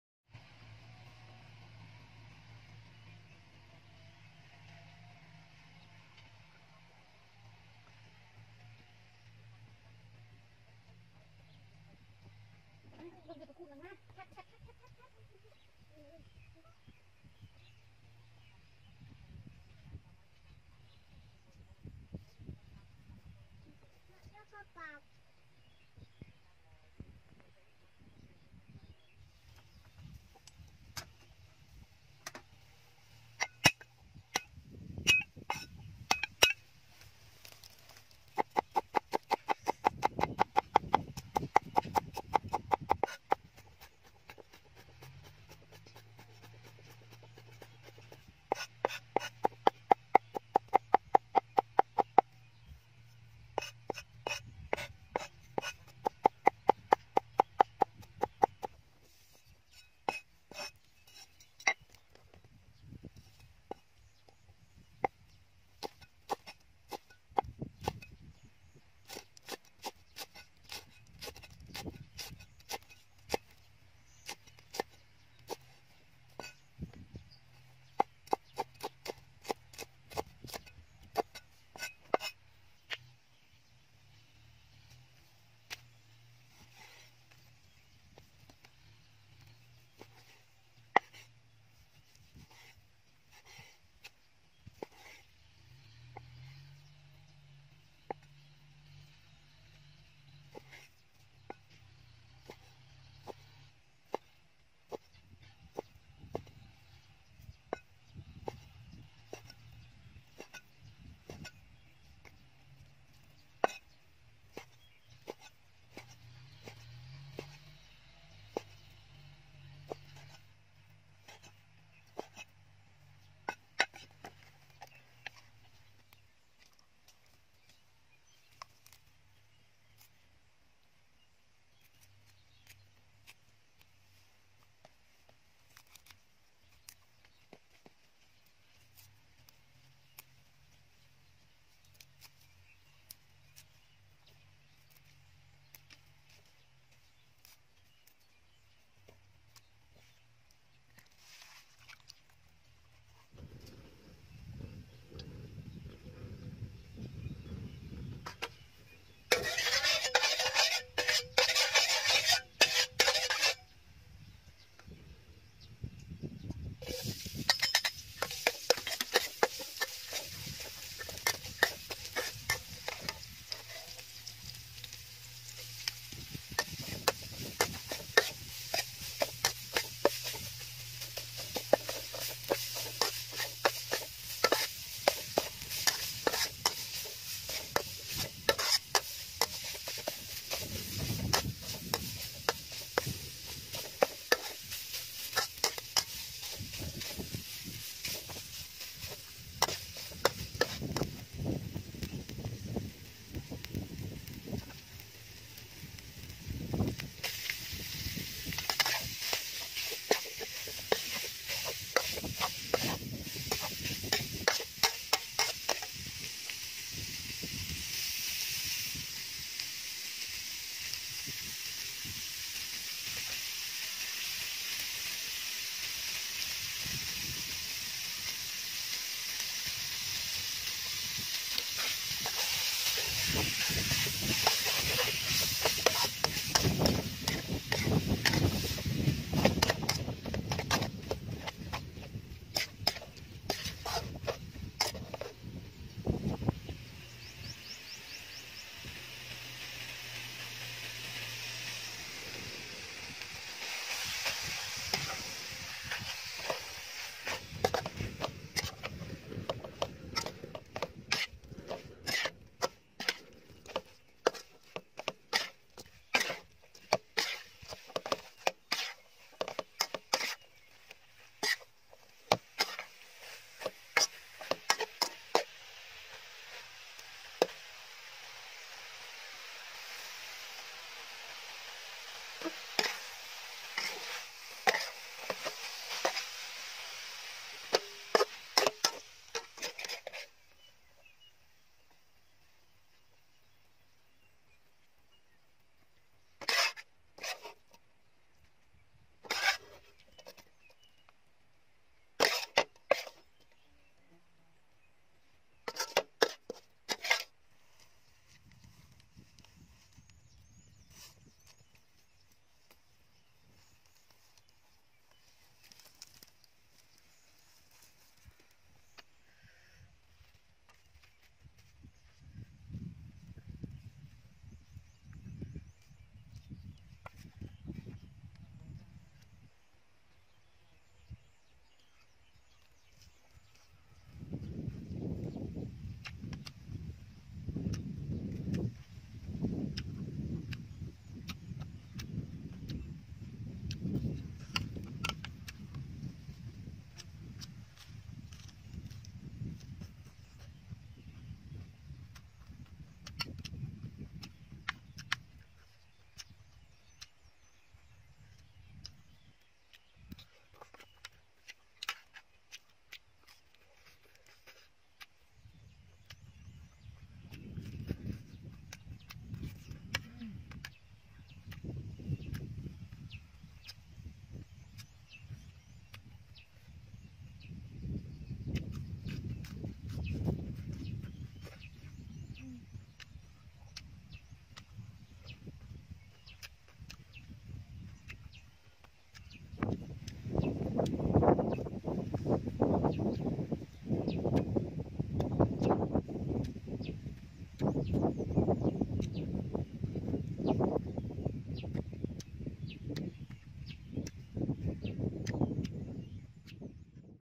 Best Video Cooking Beef With Holy basil - Eating So Delicious
- My Channel: Tv daily
- My Video:   Original (Food)

Hello all friend...
      Welcome my Tv daily channel and comeback all the videos cooking food in my country...
Today I want to show about video:
* Ingredients
- Beef
- Holy basil
- Oyster sauces
- Fish sauce
- Salt​
- Sugar
- Chili
- OMG
- Garlic